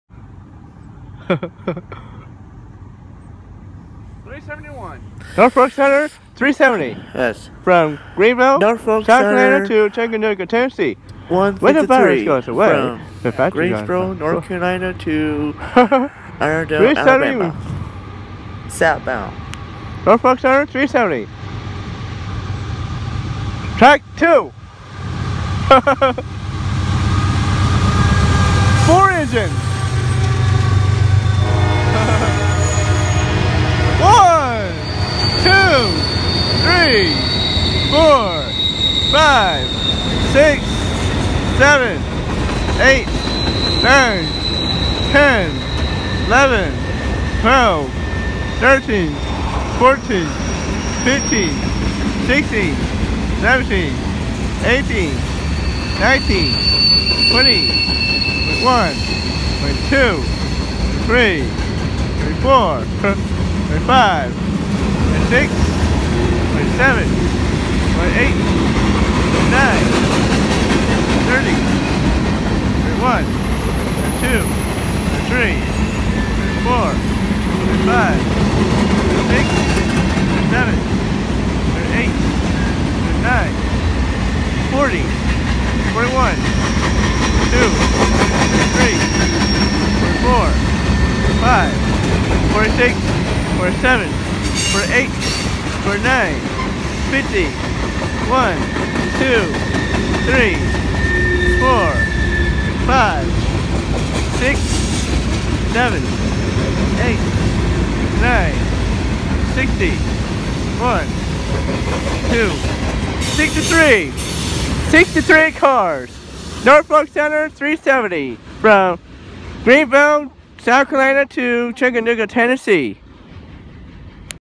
Train 3 9/7/2020 Toccoa GA
NS 153 with 4 engines and 63 cars mixed freight on Southbound Track 2 from Greensboro NC to Irondale AL local time is 2:55 pm Labor Day